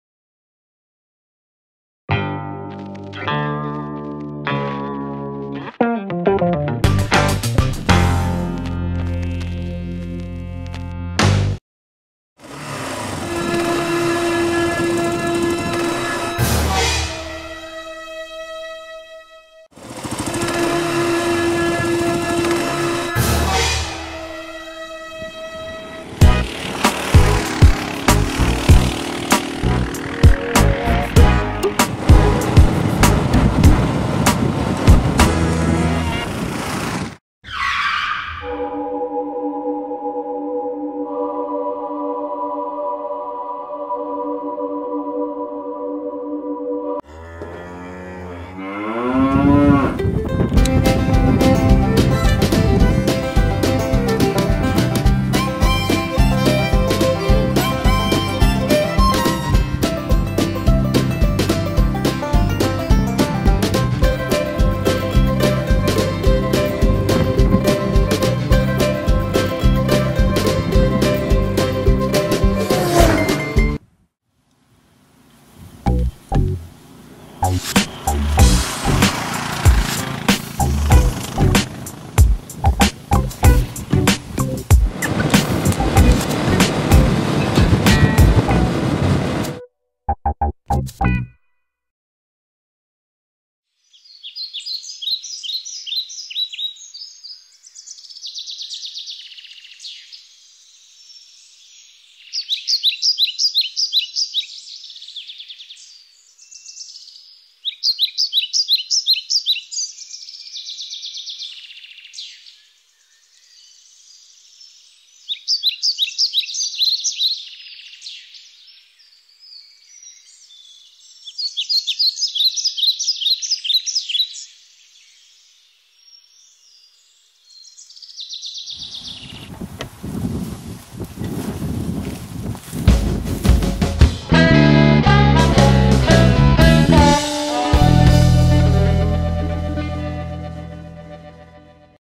Riding my KLR650 in Alberta's Kalyna Country
Riding my 2009 Kawasaki KLR650 through east-central Alberta. If you live near Edmonton, you may recognize some of this stuff.